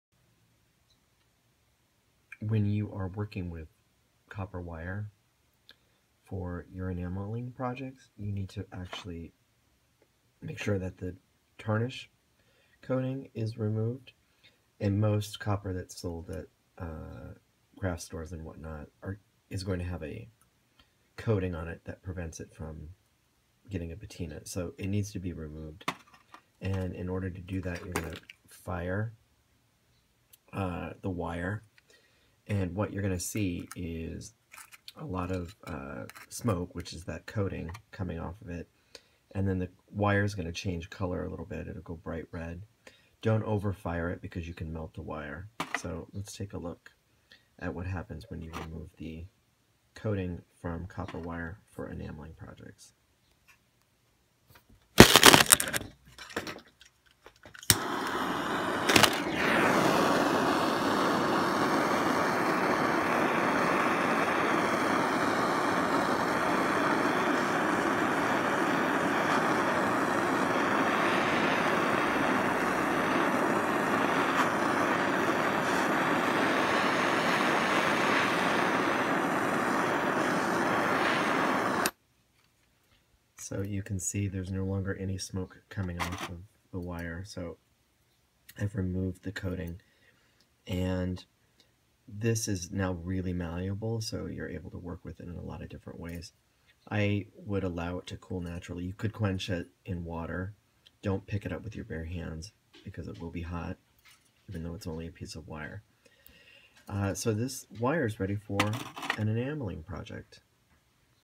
Working with Wire and Torch Enameling
There are several wire projects in Discover Torch Enameling.  Before you work with any copper wire, you must remove the tarnish coating.

Most copper wire has this type of coating and it is easily removed with a quick hit of heat from your torch!  Remember to work in a ventilated area and allow the wire to cool before you start working with it.

As a result of heat, the wire will be very malleable, making it much easier to work with as you create your project.